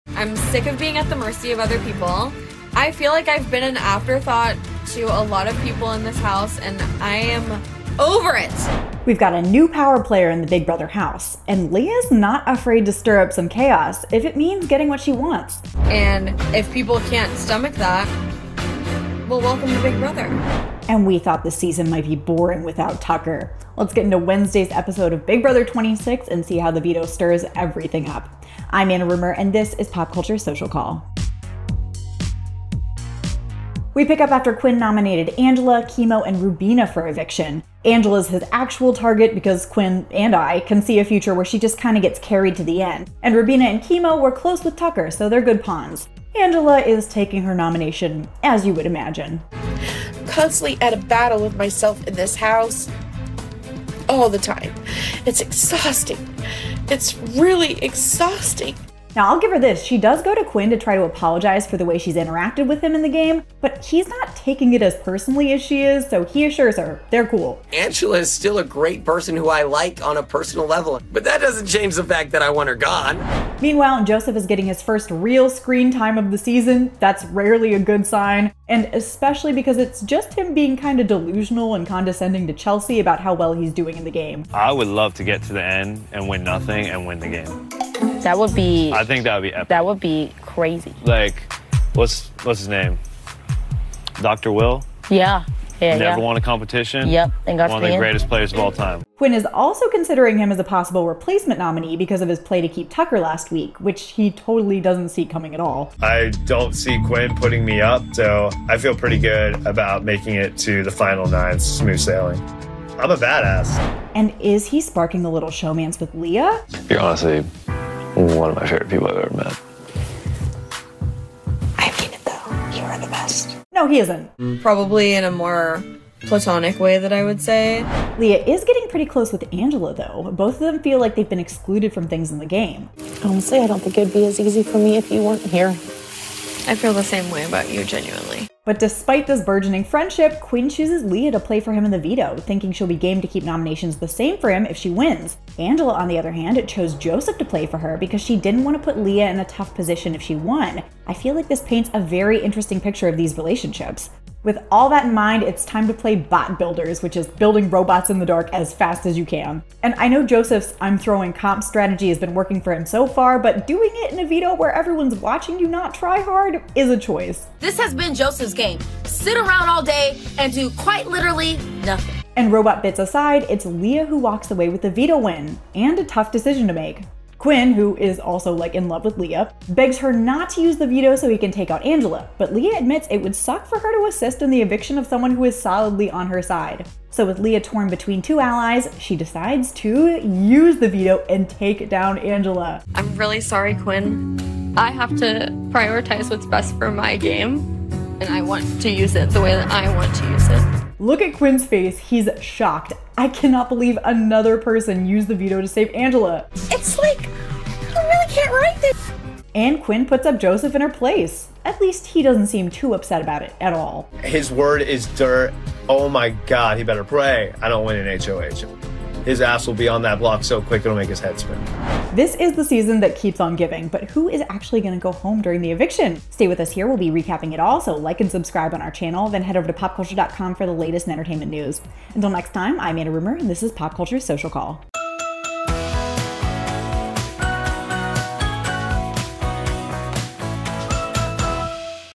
Big Brother 26: Leah BETRAYS Quinn| BB26, Episode 23 RECAP
Leah is taking control of the Big Brother 26 house - even if it means betraying her ally Quinn. See how the dramatic veto ceremony goes down as PopCulture's Social Call recaps BB26, Episode 23.